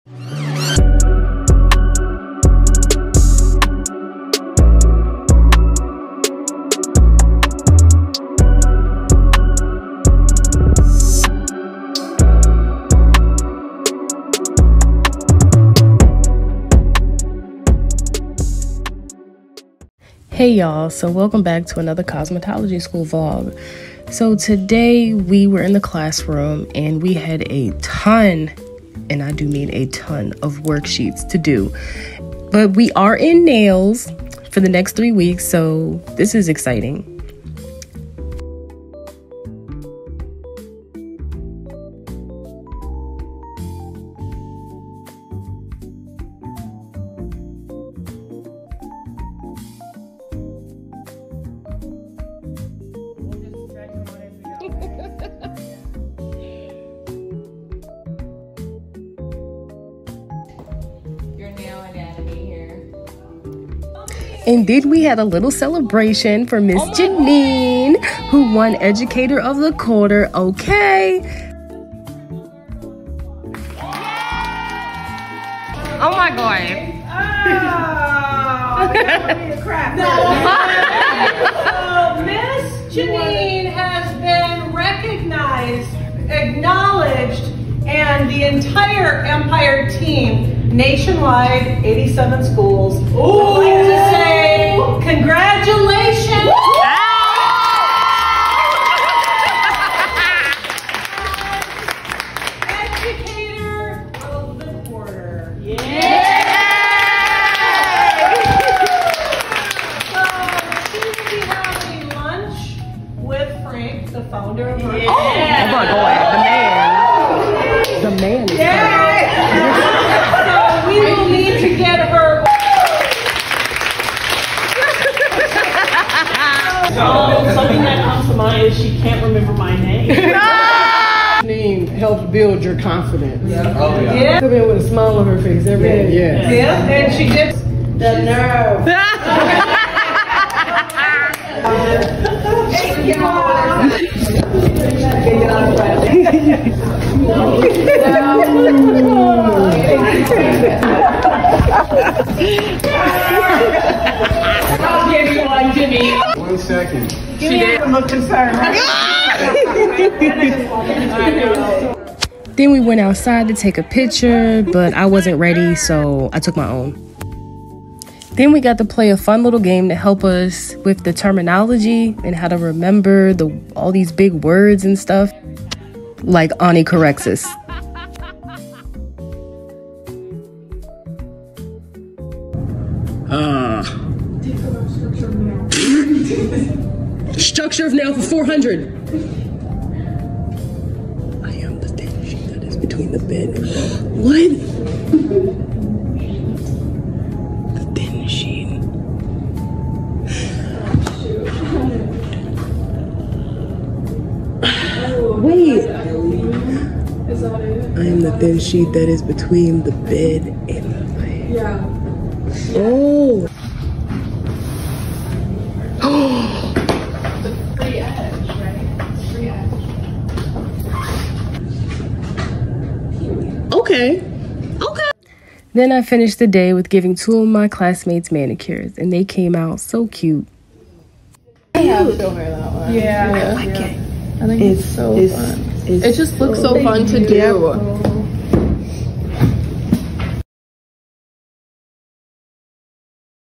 Cosmetology School Vlog | Classroom Day| Fun Nail Anatomy Review
Join me as I go through my day as a full-time cosmetology student.